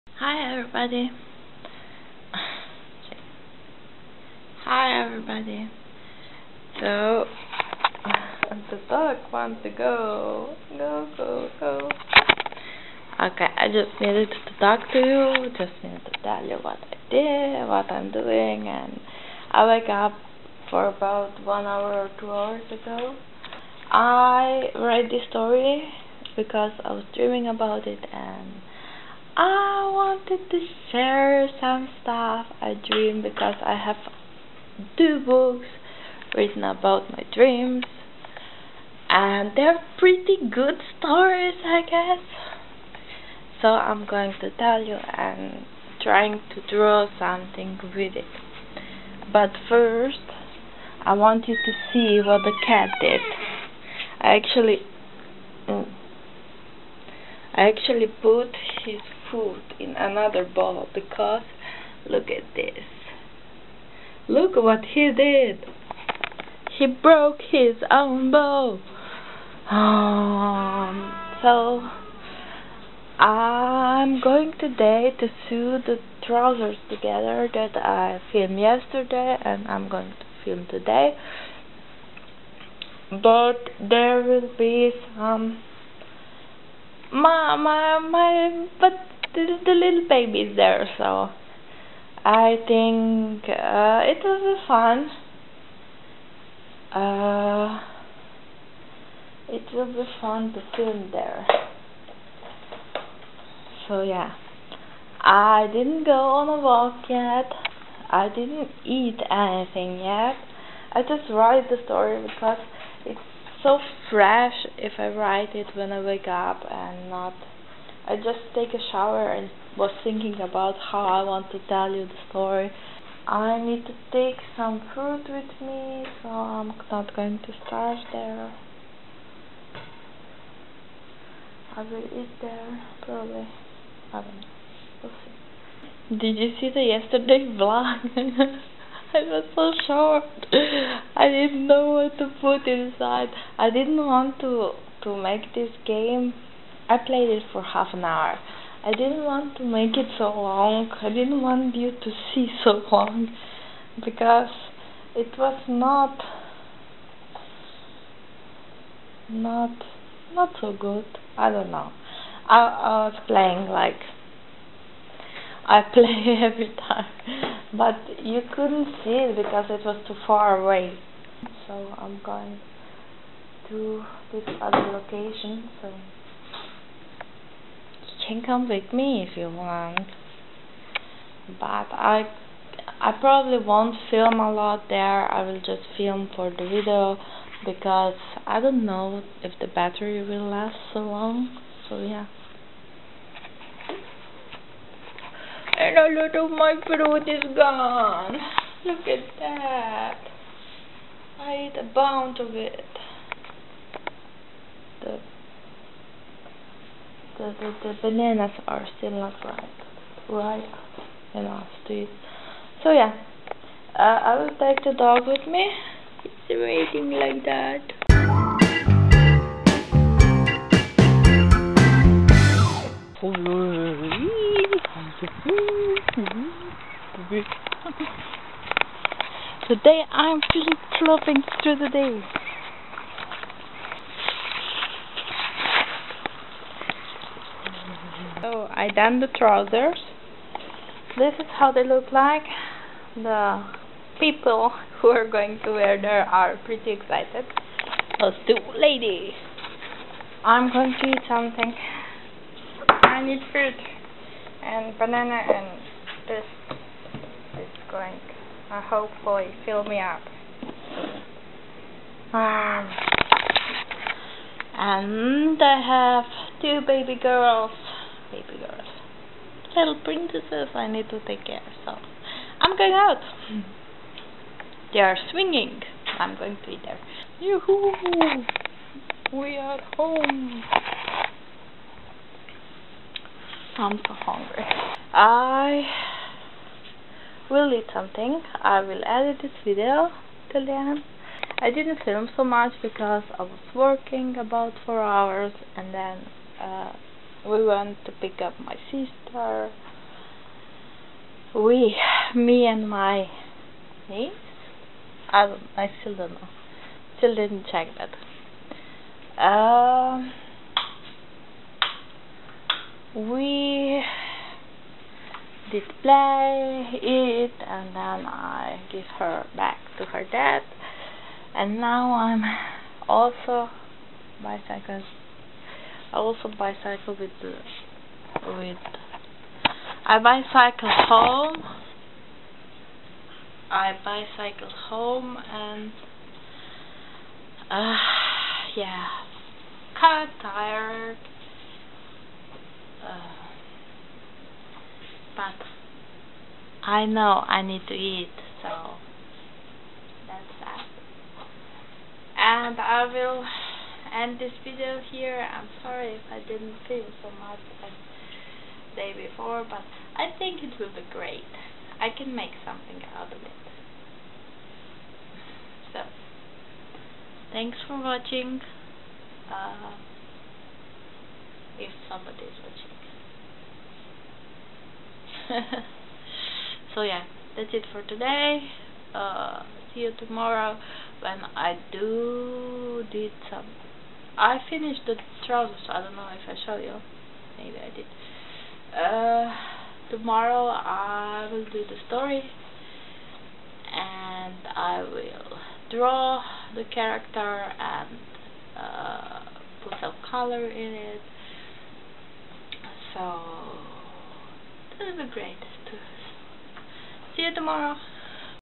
WDay 34: Vlog 5 or me talking and walking
It was just my day, with dreams, done job and children play.